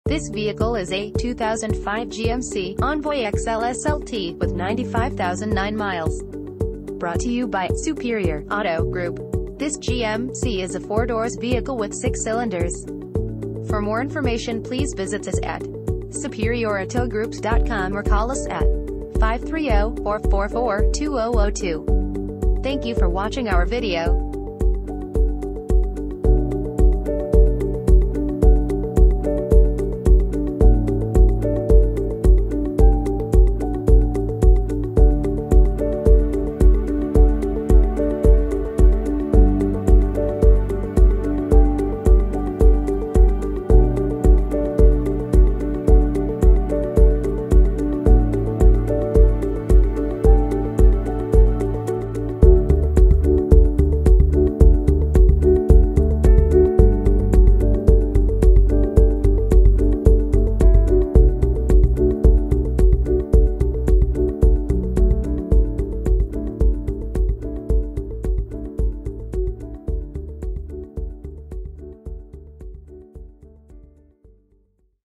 2005 GMC Envoy XL - Superior Auto Group
2005 GMC  Envoy XL

One owner vehicle, Clean title, 95,009 miles, Automatic, SUV, RWD: Rear Wheel Drive, 4 doors, 6 cylinders, Gasoline, MPG City 15 and 19 in Hwy.